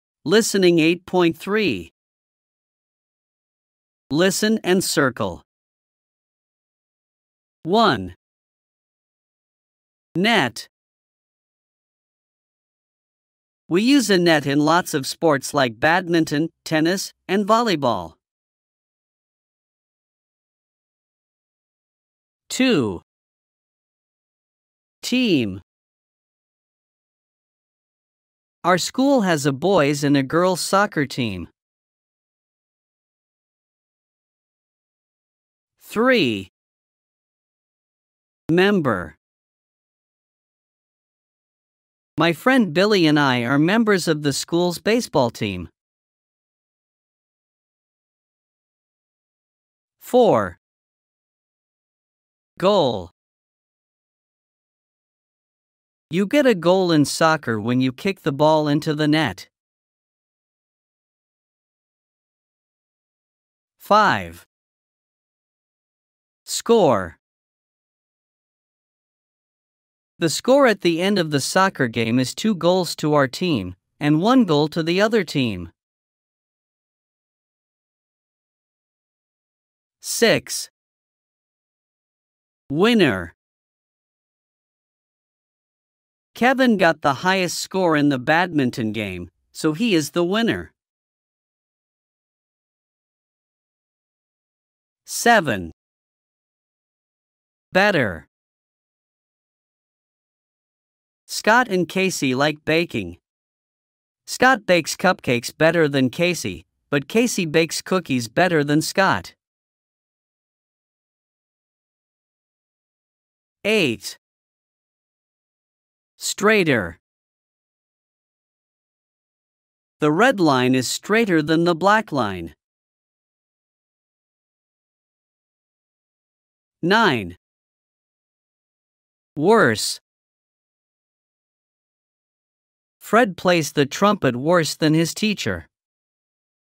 Audio Rise Up! Student Book 4 – 8.3
Hi, Sahabat ASTA
Enjoy practising your English by listening to the audio from the student book "Rise Up! Student Book Level 4 – 8.3."

Mentari Group | Bangun Pendidikan, Majukan Bangsa